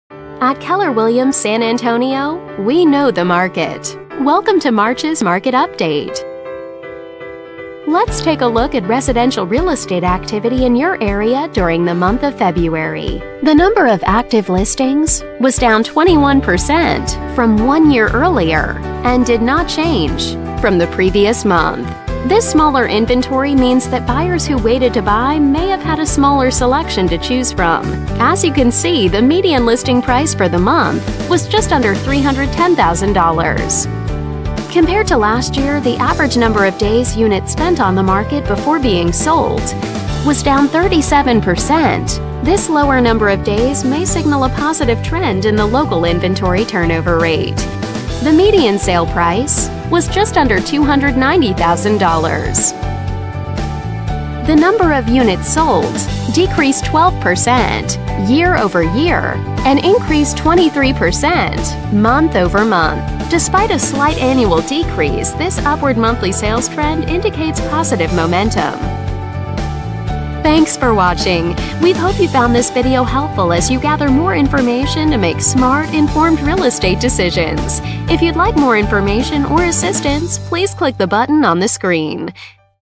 Alamo Ranch,TX, Real Estate Market Update from Keller Williams San Antonio,March, 2019
Keller Williams San Antonio Video Update recapping market activity within Alamo Ranch, TX, during the month of March, 2019